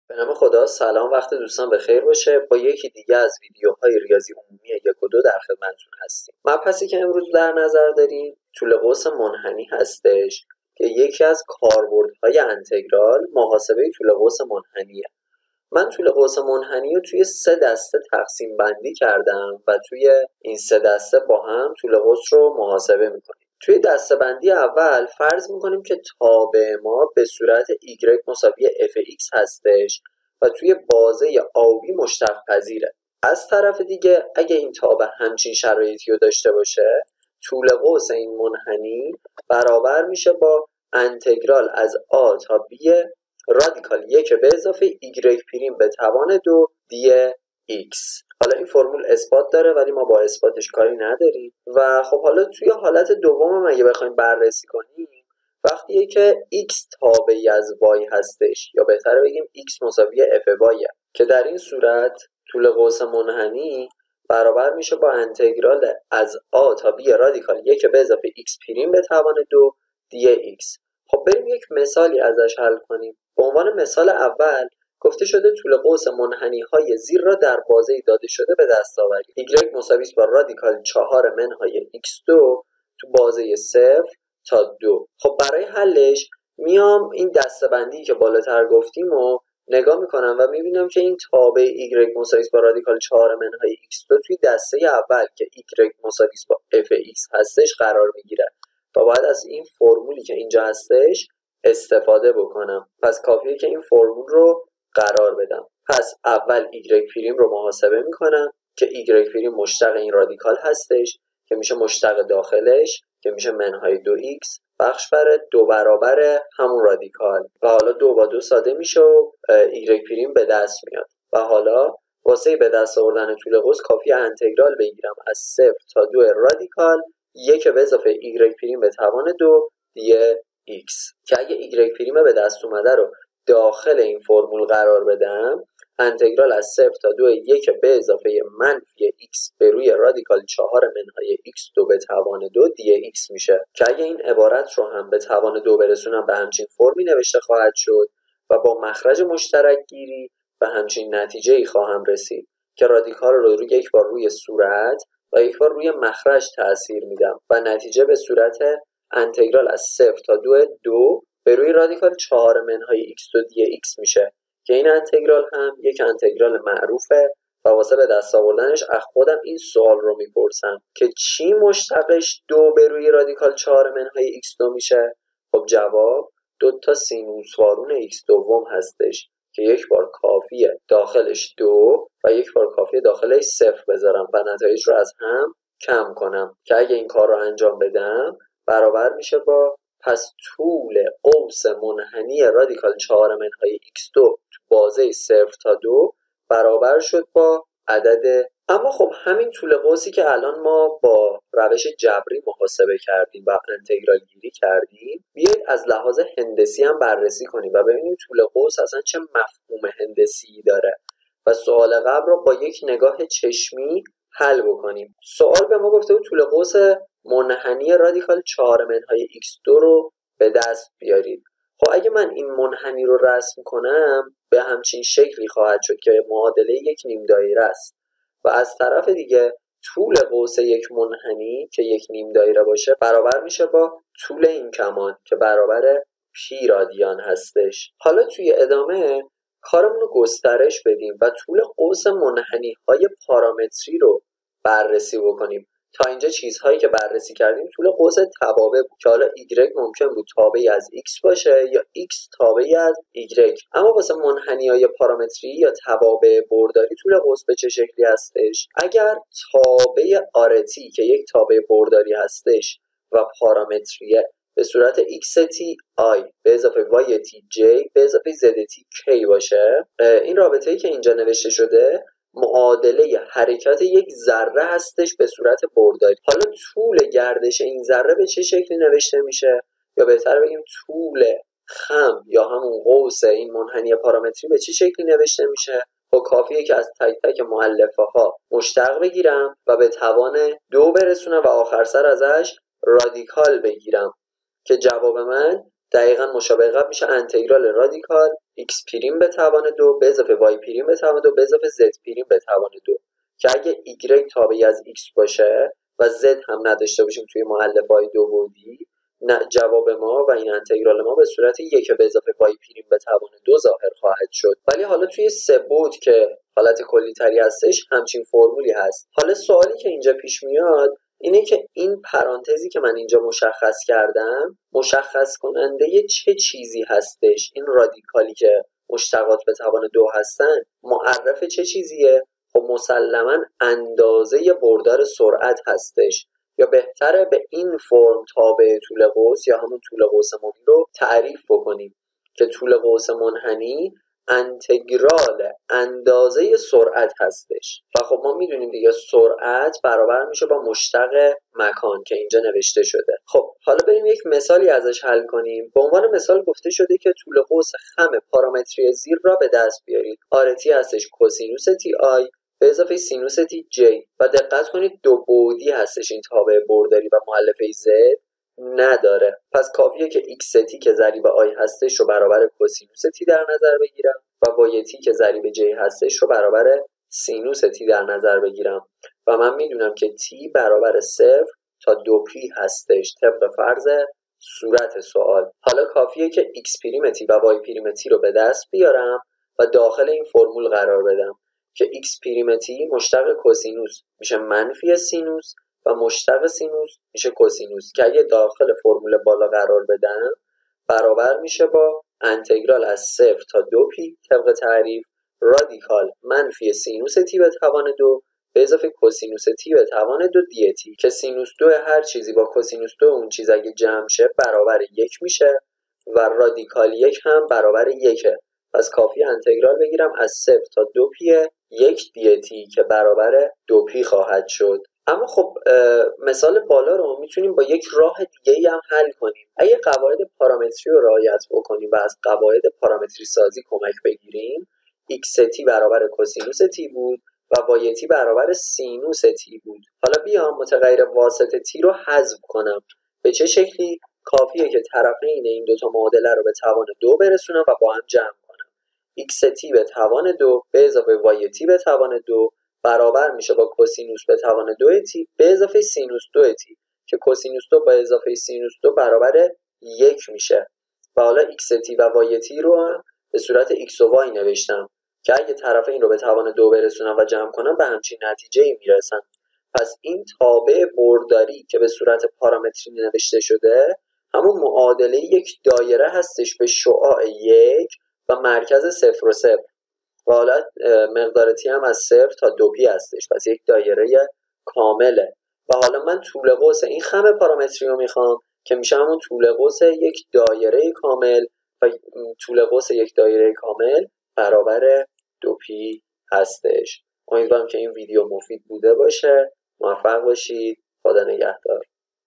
طول قوس منحنی
در این ویدیو با نحوه محاسبه طول قوس منحنی آشنا می شوید